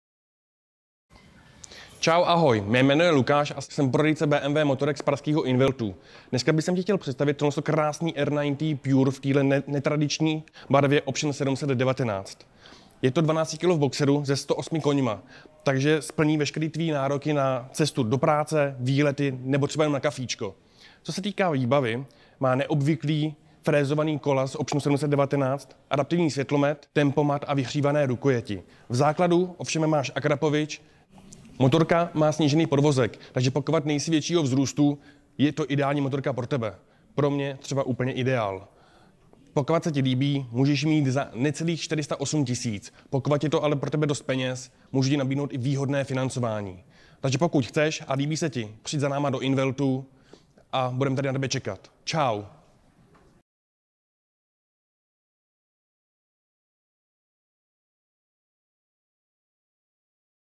BMW R nineT Pure Option 719
BMW R nineT Pure má silný vzduchem/olejem chlazený motor, na asfaltu se postará o okamžiky, při nichž ti naskočí husí kůže. Klasický vzhled roadsteru s kompaktní nádrží a štíhlou zadní částí připomíná počátky značky BMW Motorrad. Benzínový motor s výkonem 80/109 kW/HP, toč. momentem 70 kW (95 PS) při 7 000 ot./min. a spotřebou 5,1 l / 100 km reaguje přesně a citlivě. Existuje mnoho možností individualizace. Vytvoř si zcela osobitý motocykl. Popusť uzdu své fantazii!